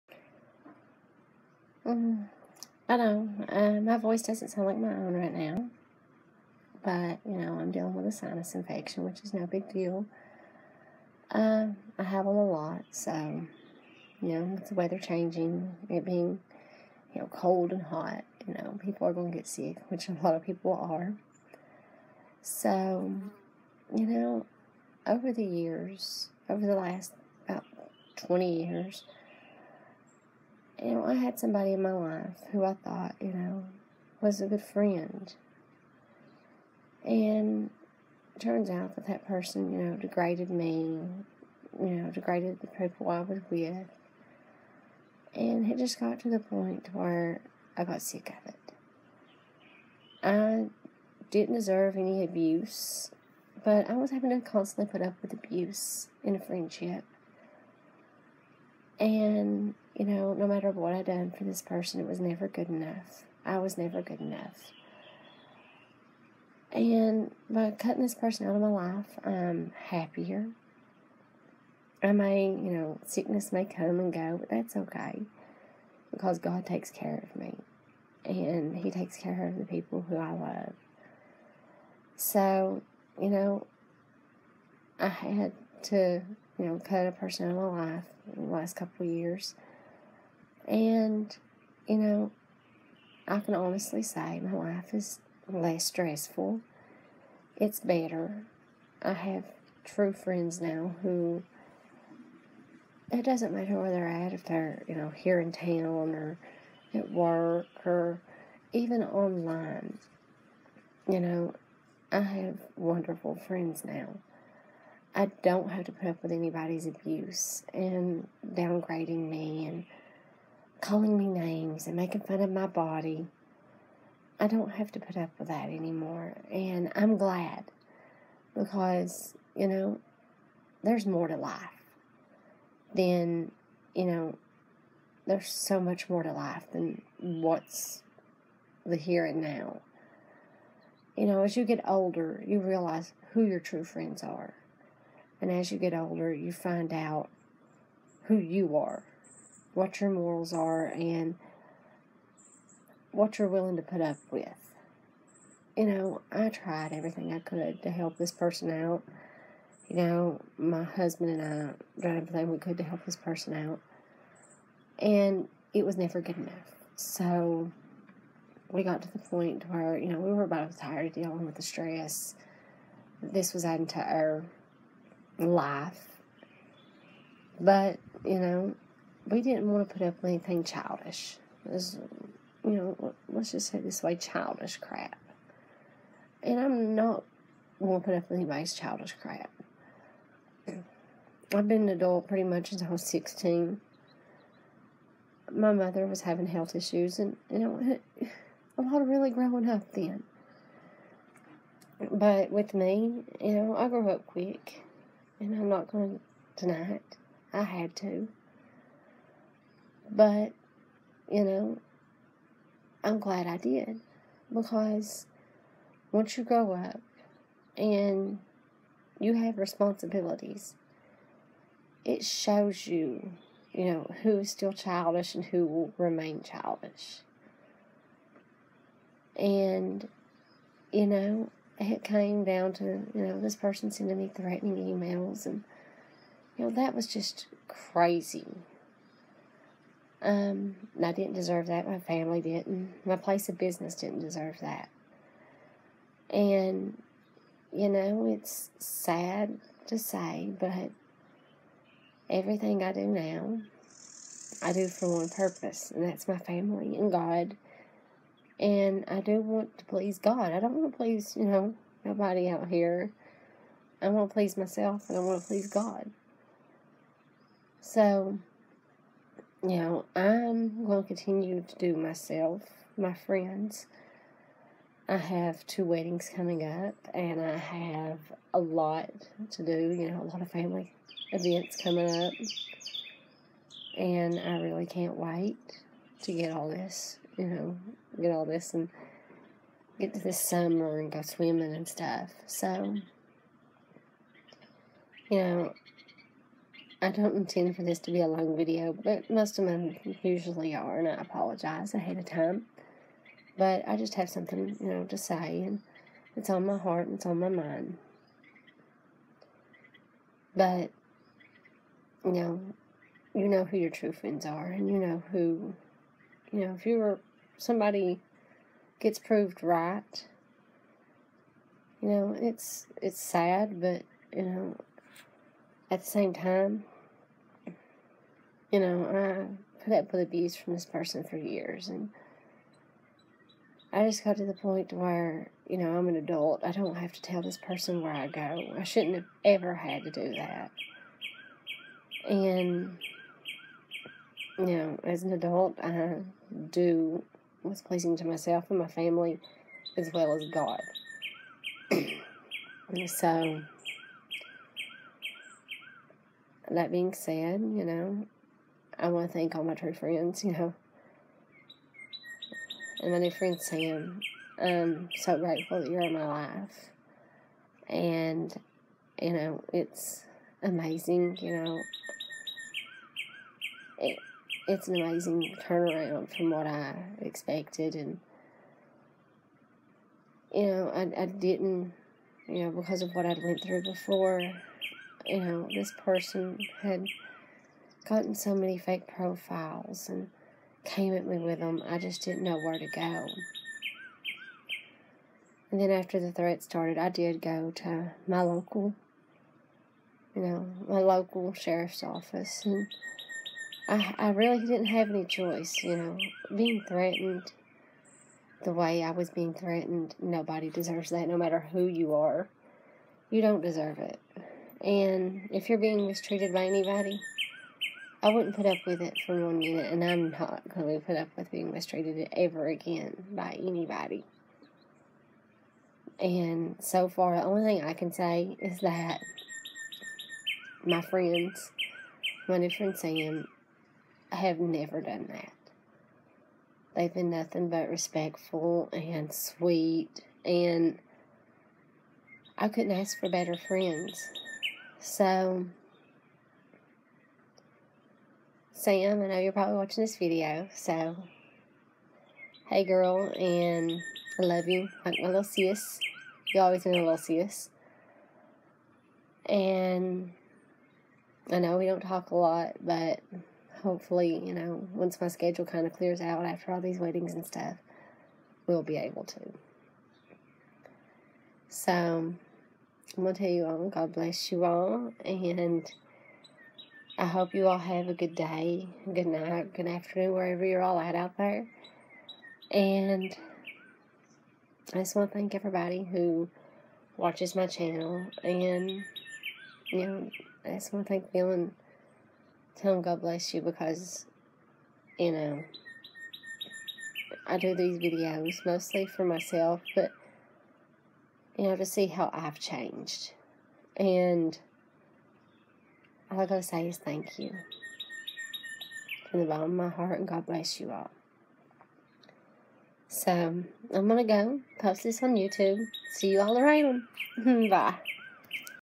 Growing up , we always learn who our true friends are.
When we grow up and become responsible adult, we can always see who the childish people are. No one deserves to be abused by anyone. Sorry for my cold, but I had to get some things off my chest. Love you sissy , Samantha Pearson , you are awesome.